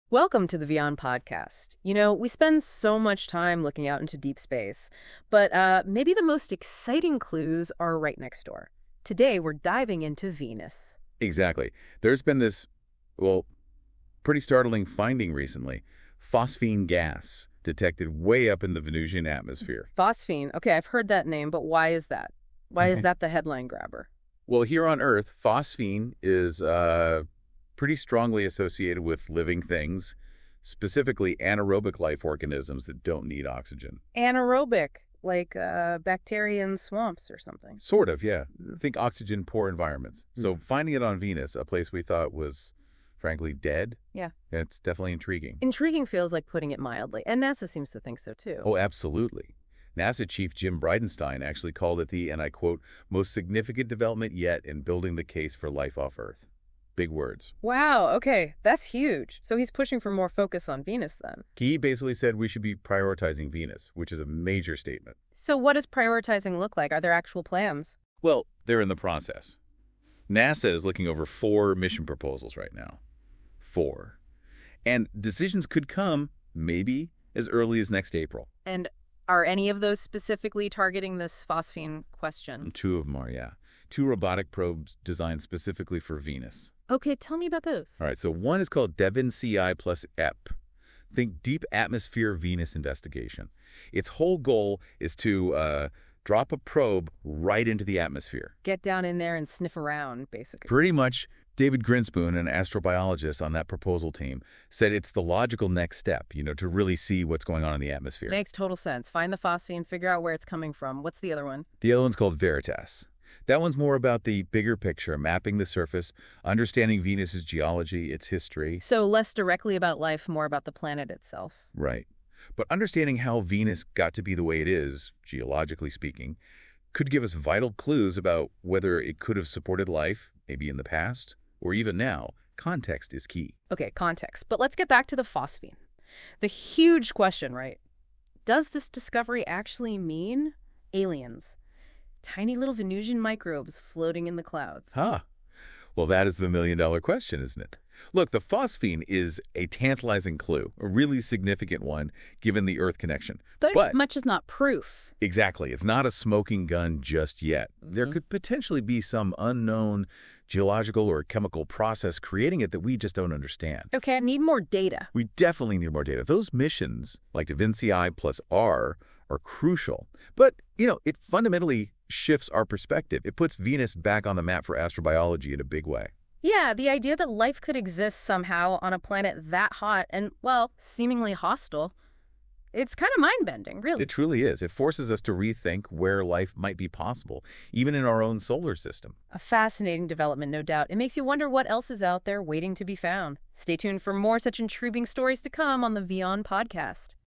Venus Just Got Interesting – Could Life Be Floating in Its Atmosphere? | WION Podcast
A stunning discovery has put Venus back in the spotlight—phosphine gas has been detected in its atmosphere. On Earth, this gas is strongly linked to anaerobic life, raising big questions about possible microbial activity on our neighboring planet. NASA is now reviewing missions like DAVINCI+ and VERITAS to dig deeper into this mystery. Could Venus, long thought to be lifeless, actually host life in its clouds? This finding could reshape how we search for life beyond Earth.